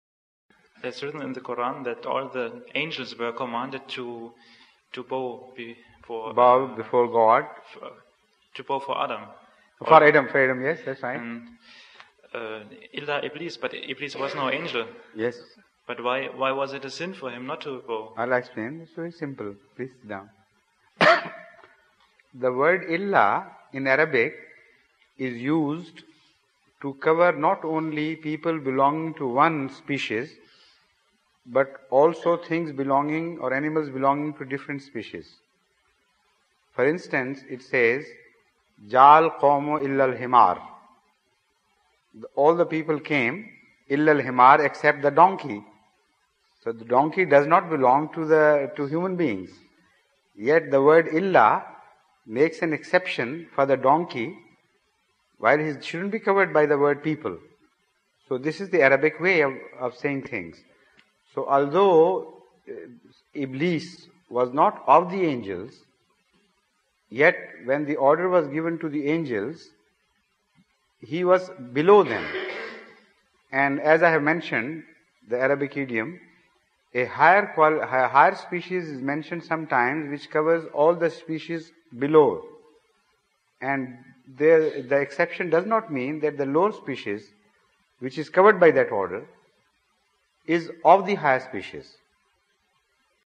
Why was it considered a sin for Iblis not to bow to Adam, as the angels were commanded to do.....?
Why was it considered a sin for Iblis not to bow to Adam, as the angels were commanded to do, since he was not an angel?